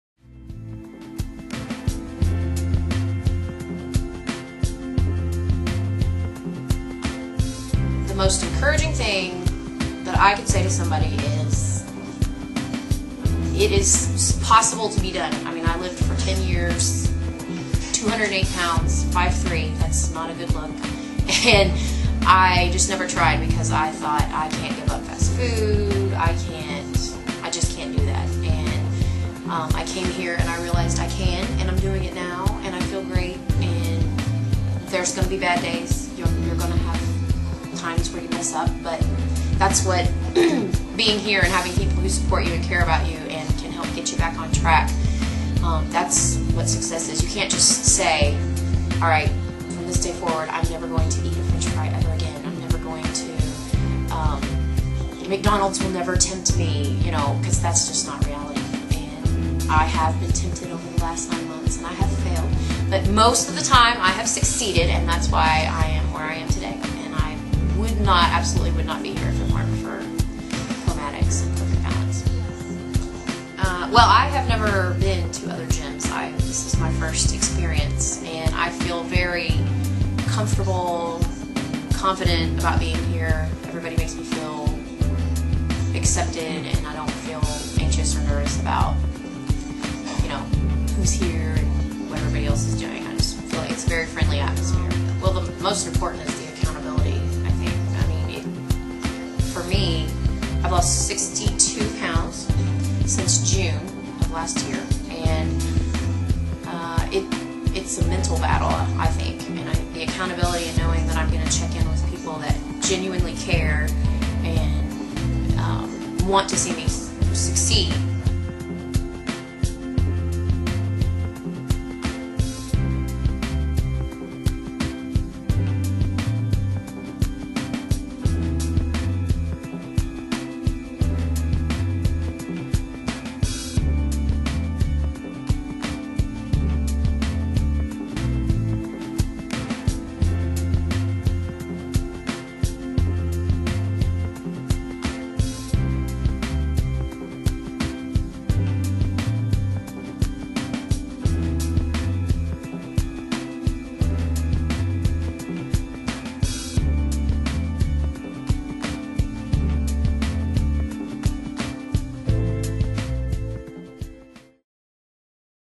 Shannon Smith results.wmv
Shannon Smith has lost over 65lbs using the Perfect Balance Nutritrax program. Watch the effect it has had on her family and experience her journey. She is very encouraging to anyone that would like to lose weight.